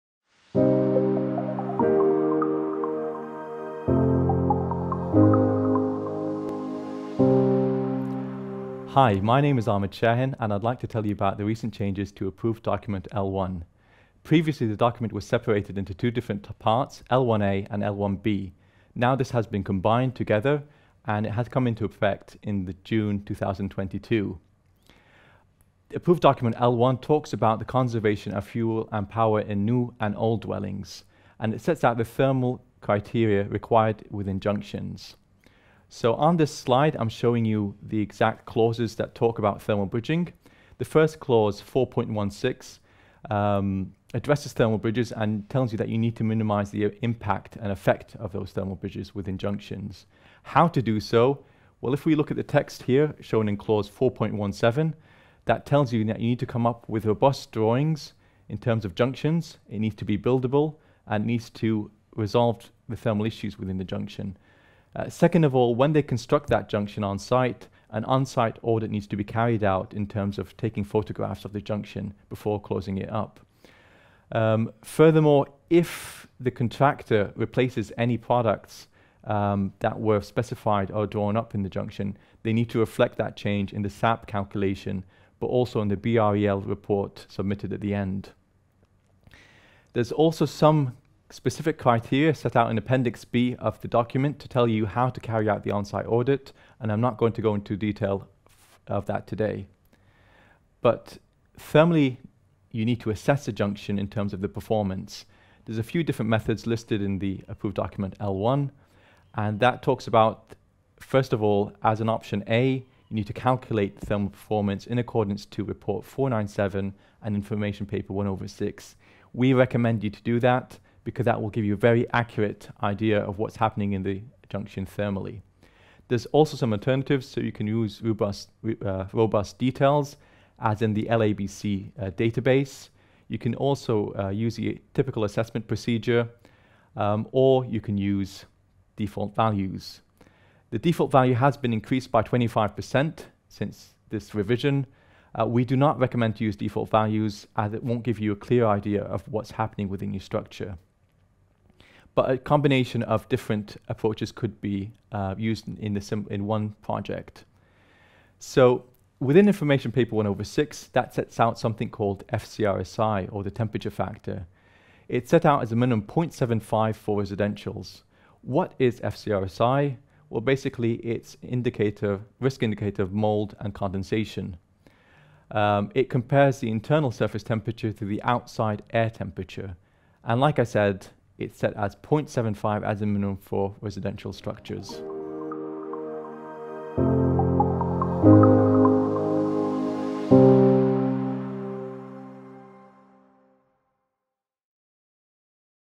Thermal Bridging in Construction - Changes to Approved Document L1
Learn about the recent changes involving thermal bridging in  Approved Document L1, the supporting document which provides guidance on the ways to meet the requirements to Building Regulation Part L - Conservation of fuel and power.